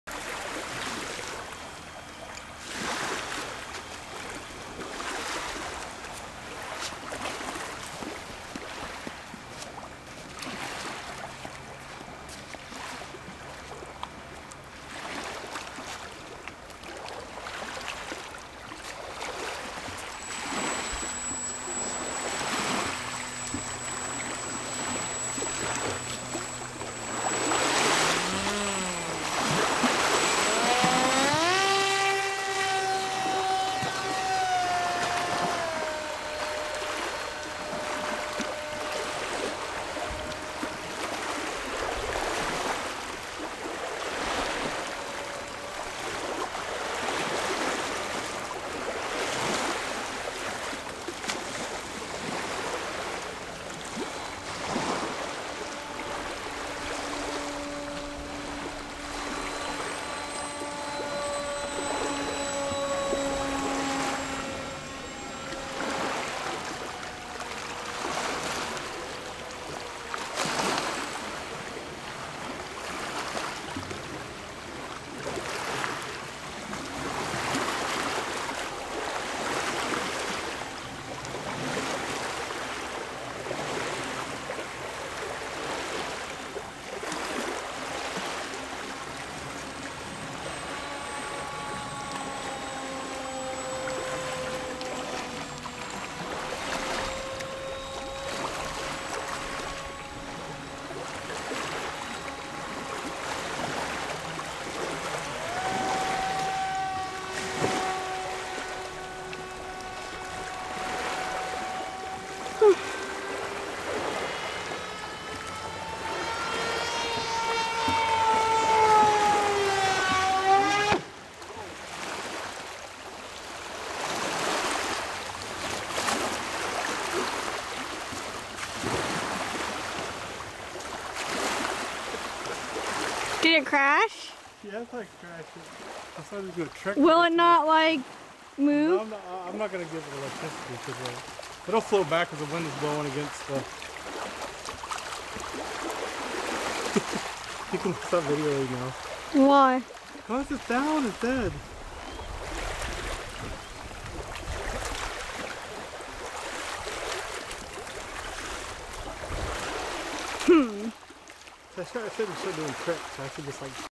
Icon A5 RC plane flying over Neuse River
Flying then crashing my Icon A5 RC plane in the Neuse